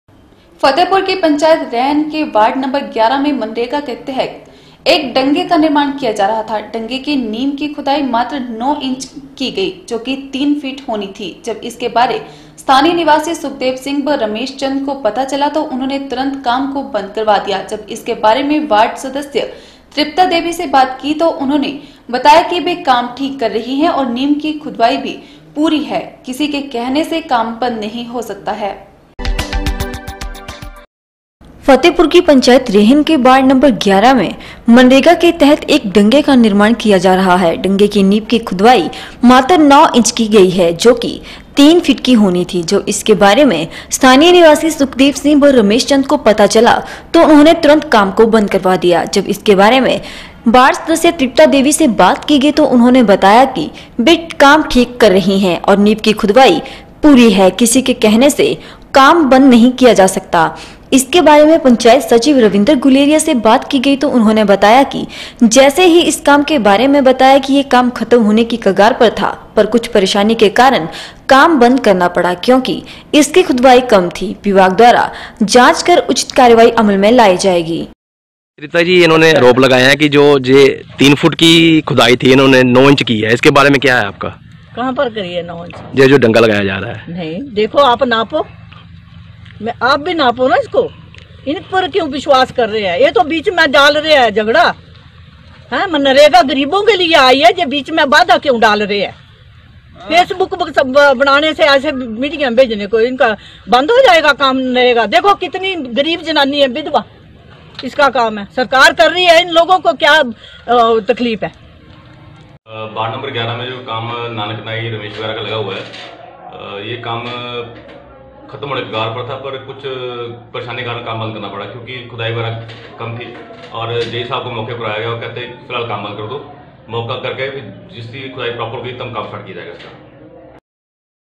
11 FEB N 11 फतेहपुर की पँचायत रेहन के वार्ड नं 11 में मनरेगा के तहत एक डंगे का निर्माण किया जा रहा
फतेहपुर की पँचायत रेहन के वार्ड नं 11 में मनरेगा के तहत एक डंगे का निर्माण किया जा रहा है।डंगे के नींव की खुदवाई मात्र 9 इंच की गई,जोकि 3 फिट होनी थी।जब इसके वारे स्थानीय निवासी सुखदेव सिंह व रमेश चंद को पता चला तो उन्होंने तुरंत काम को बंद करवा दिया। जब इसके बारे में वार्ड सदस्य तृप्ता देवी से बात की तो उन्होंने बताया कि वे काम ठीक कर रही है और नींव की खुदवाई भी पूरी है। किसी के कहने से काम बंद नही हो सकता।